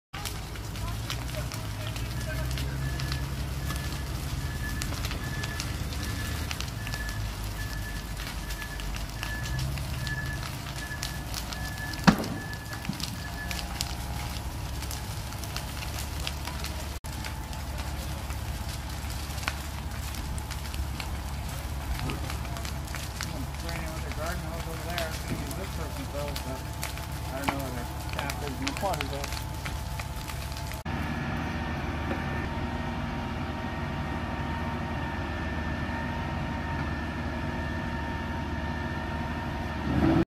Fire in Wawanesa
Heard a couple of pops and looked out to see a garage on fire just down our back lane. The garage was destroyed, but thankfully the fire department got it under control quickly. No one was hurt.